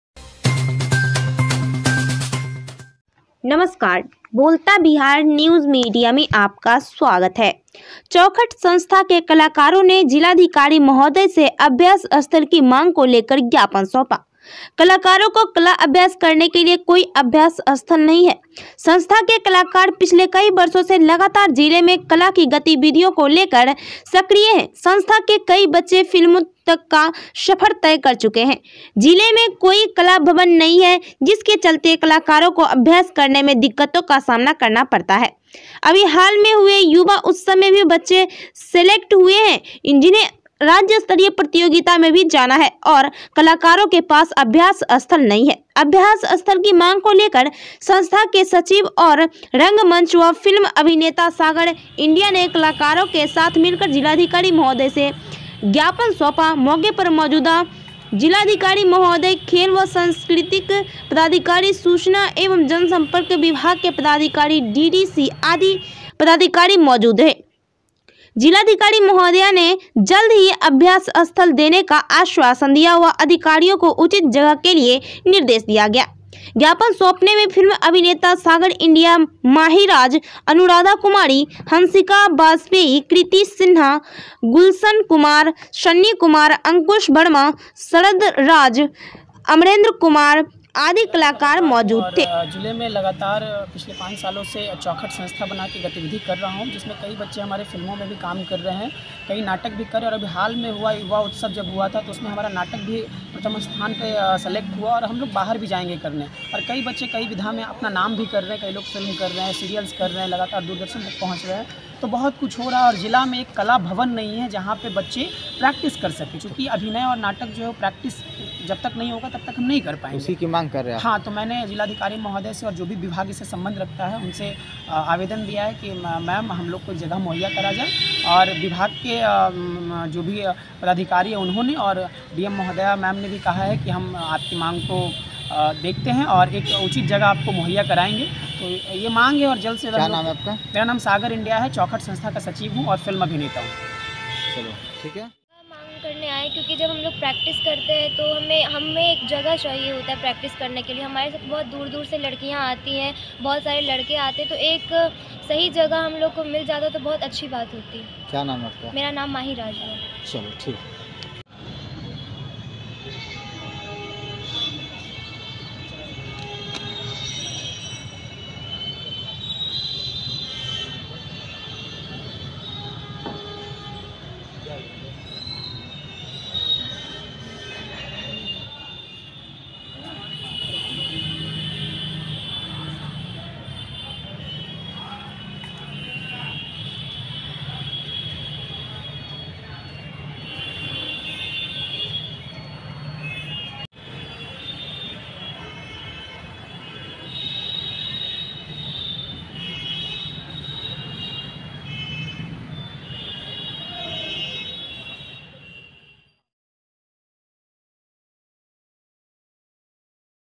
चौखट संस्था के कलाकारों ने, जिलाधिकारी महोदय से अभ्यास स्थल की मांग को लेकर, ज्ञापन सौंपा
लोकेशन नवादा
नवादा से अनिल शर्मा के साथ कैमरामैन प्रिंस कुमार की रिपोर्ट

कलाकारों को कला अभ्यास करने के लिए कोई अभ्यास स्थल नहीं है।

संस्था के कलाकार पिछले कई वर्षों से, लगातार जिले में, कला की गतिविधियों को लेकर सक्रिय हैं, संस्था के कई बच्चे फिल्मों तक का सफर तय कर चुके है, जिले में कोई कला भवन नहीं है, जिसके चलते कलाकारों को अभ्यास करने में दिक्कतो का सामना करना पड़ता है। अभी हाल में हुए युवा उत्सव  में भी बच्चे सेलेक्ट हुए हैं, इन्हें राज्यस्तरीय प्रतियोगिता  में भी जाना है, और कलाकारों के पास अभ्यास स्थल नहीं है अभ्यास स्थल की मांग  को लेकर संस्था के सचिव और रंगमंच
व फिल्म अभिनेता सागर इंडिया ने कलाकारों के साथ मिलकर जिलाधिकारी महोदया को ज्ञापन सौंपा, मौके पर मौजूदा जिलाधिकारी महोदय  खेल व सांस्कृतिक पदाधिकारी, सूचना एवं जनसंपर्क विभाग के पदाधिकारी डीडीसी आदि पदाधिकारी मौजूद रहे, जिलाधिकारी महोदया ने, जल्द ही अभ्यास स्थल देने का आश्वासन दिया व अधिकारियों को उचित जगह के लिए निर्देश दिया।

ज्ञापन सौंपाने में, फिल्म अभिनेता सागर इंडिया, मही राज, अनुराधा कुमारी, हंसिका बाजपेई, कृति सिंहा, गुलशन कुमार, सन्नी कुमार, अंकुश वर्मा, शरद, राज, अमरेंद्र कुमार, आदि कलाकार मौजूद रहे